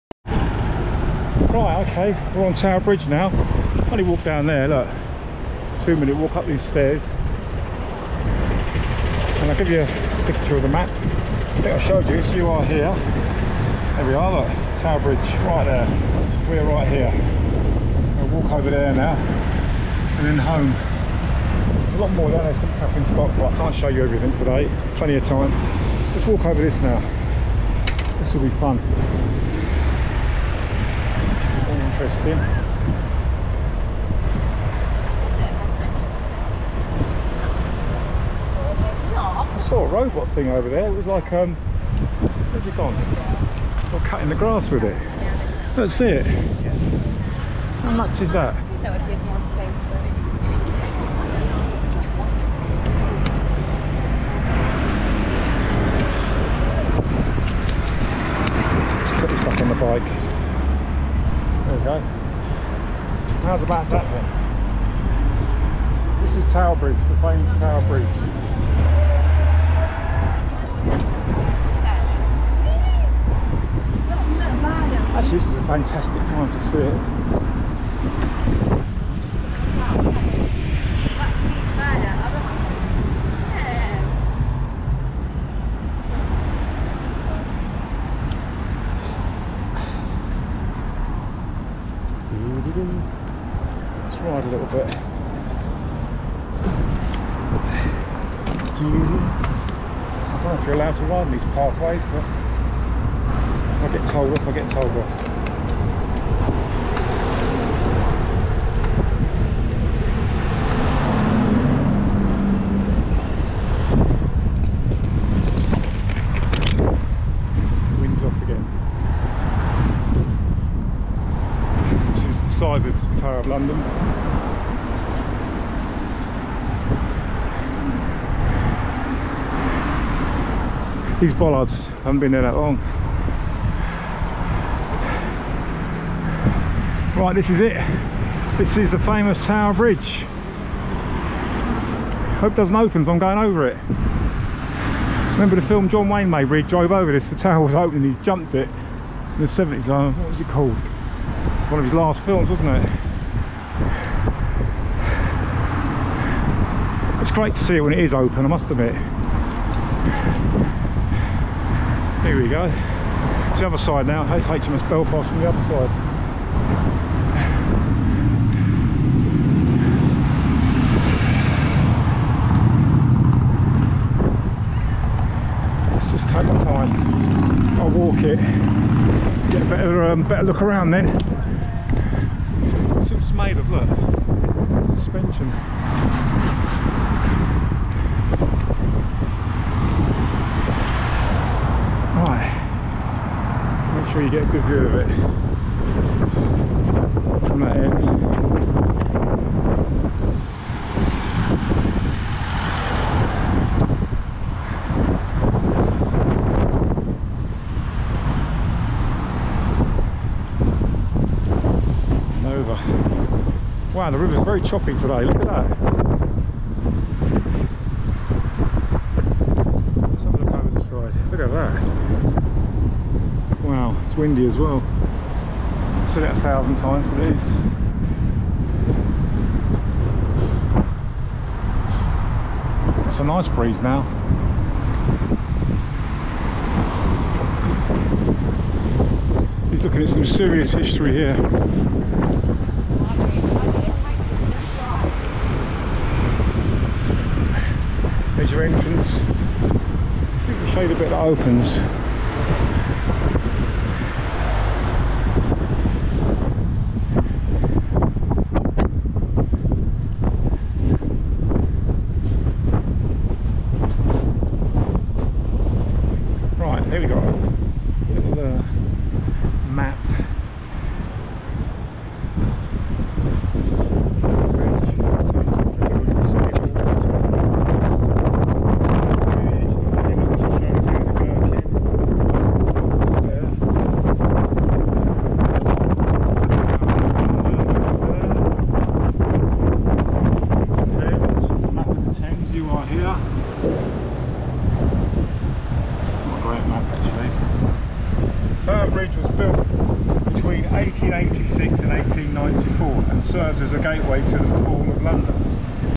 bike ride over tower bridge London amazing ,view over bridge to battle ship Hms Belfast !
just a quick look at tower bridge to see how busy it is   today very windy !  sorry bout sound quality will film again after lock down   other clips to come from this film borough market ,London bridge ,the clink prison Thames, Hms Belfast  the fire of London monument